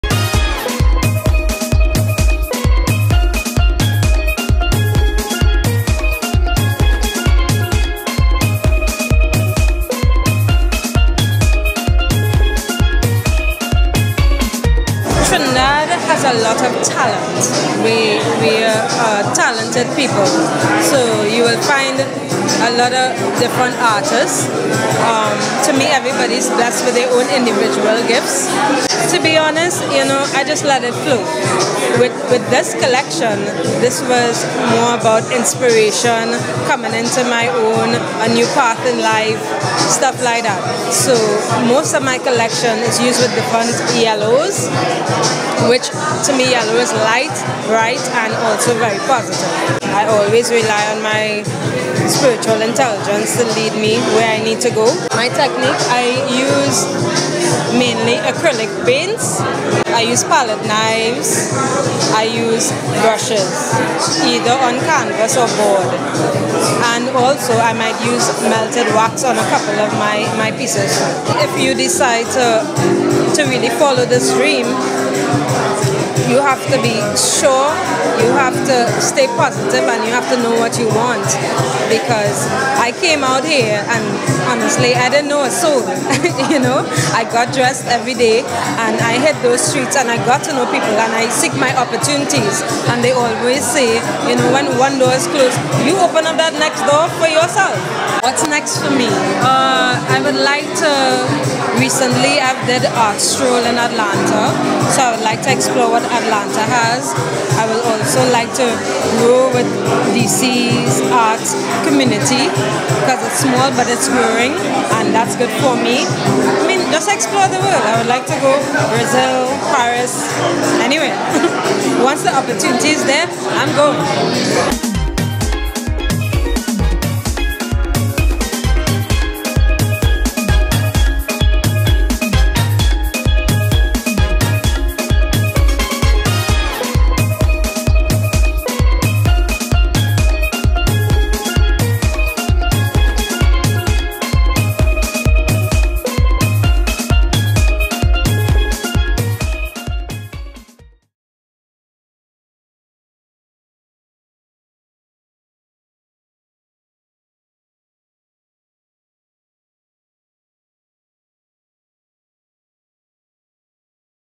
Artist Spotlight: Andrea McKenzie
Expressionist bold coloured paintings for personal spaces